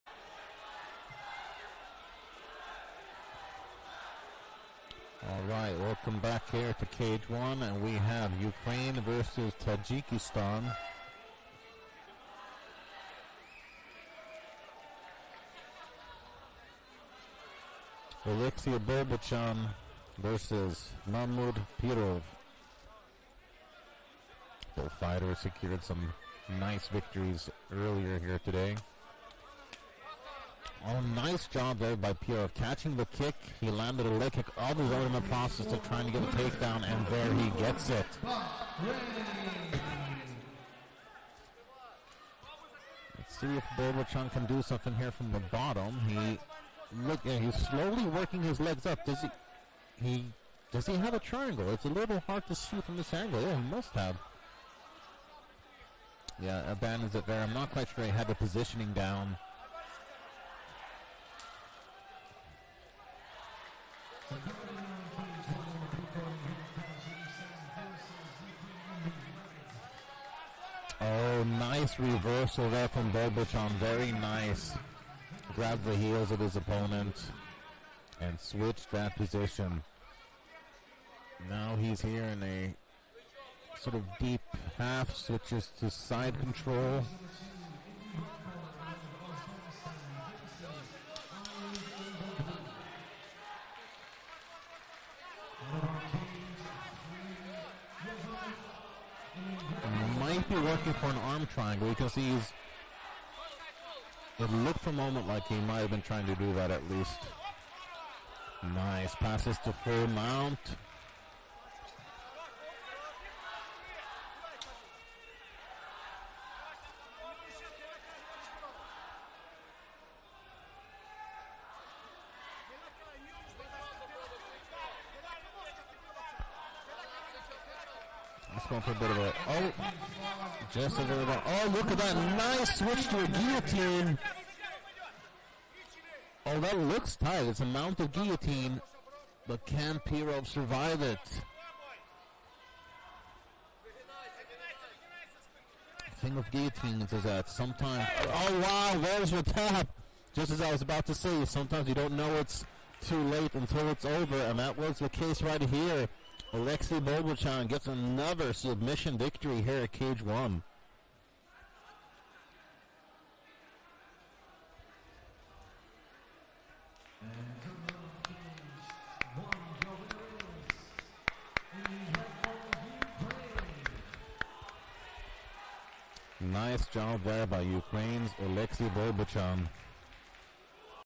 VS Oleksii Bolbochan Ukraine VS Mahmud Pirov Tajikistan immaf tv
ЧЕМПИОНАТИ ҶАҲОН БАЙНИ НАВРАСОН ММА АБУ ДАБИ 2022